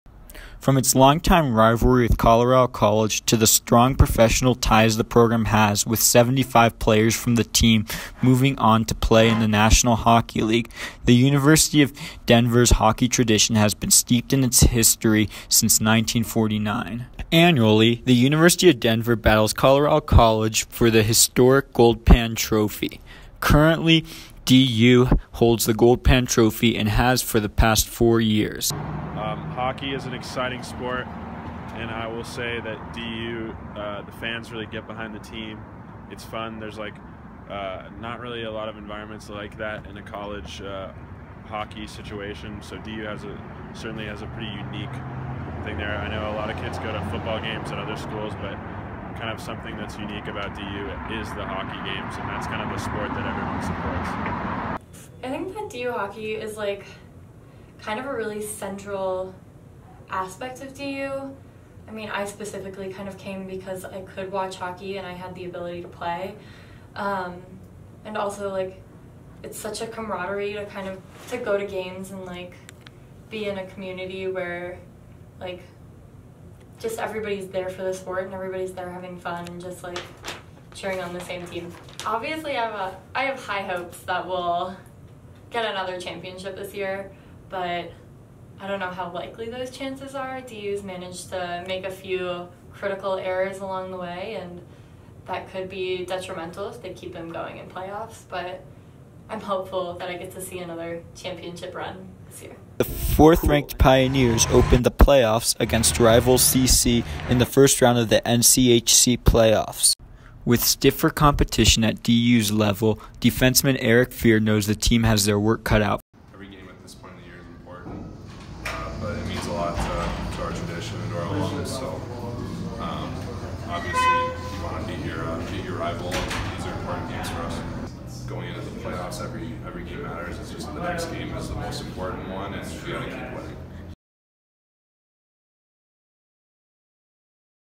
University of Denver Hockey
Since 1949 from its rivalry with Colorado College to the eight national championships the University of Denver has had one of the richest college hockey traditions in the country